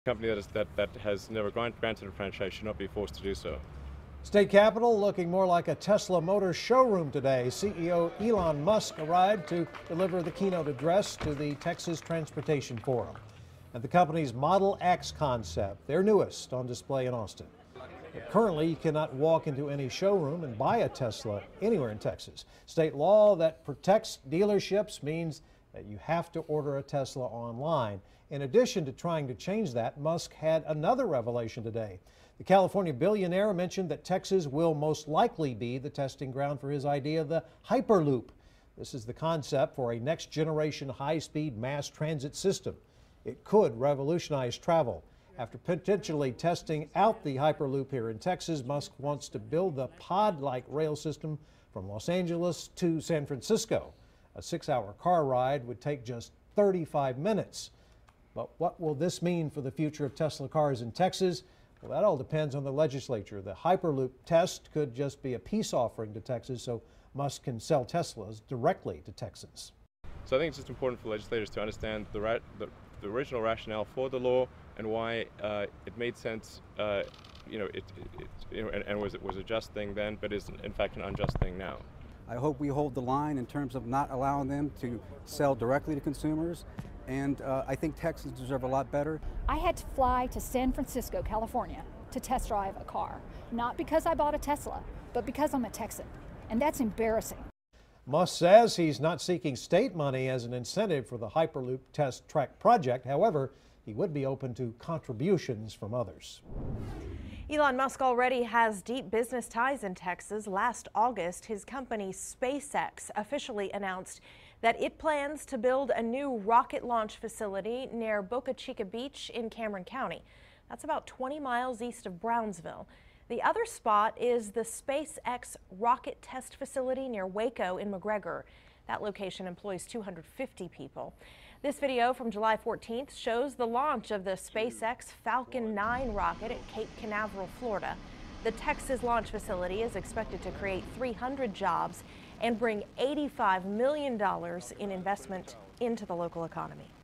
Elon Musk touts Texas for speed-of-sound transit test site
Entrepreneur Elon Musk says Texas is the leading contender for a five-mile-long test track for his conceptual Hyperloop project.